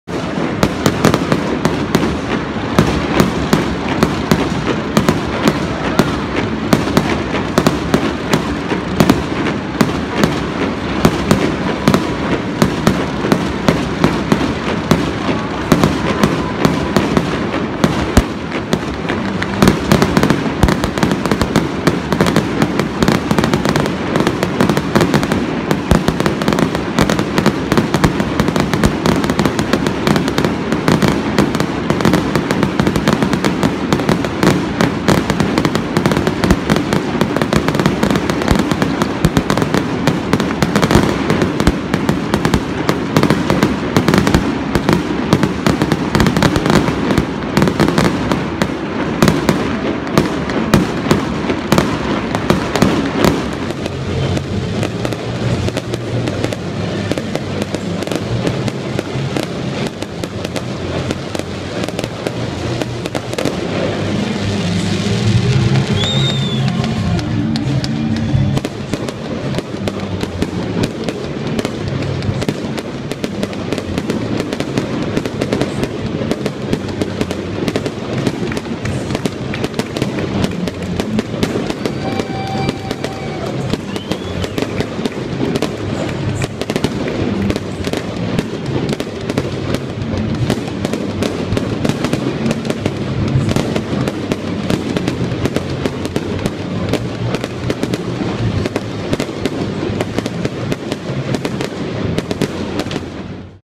Detroit Tigers Fireworks - Comerica Park
For those of you missing the Friday night fireworks at the Tiger games!